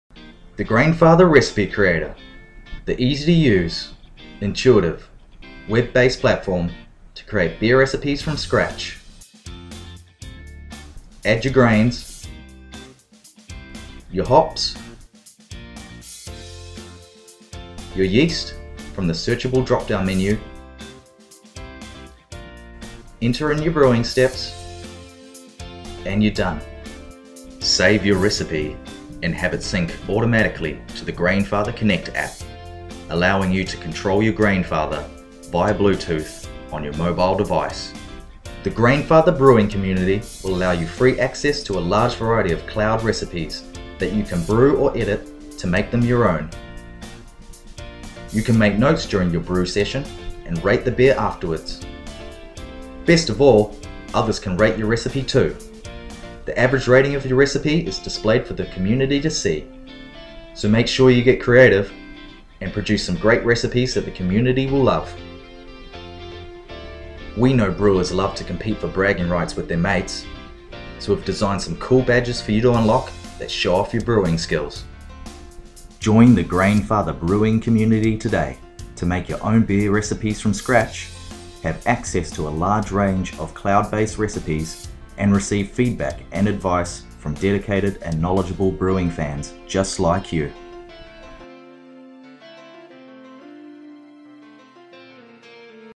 Grainfather Brewing Community
Join the Grainfather Brewing Community today to make your own beer recipes from scratch (which you can sync directly to the Grainfather Connect App - not currently available for Android, coming soon), have access to a large range of cloud base recipes and receive feedback and advice from dedicated and knowledgable brewing fans!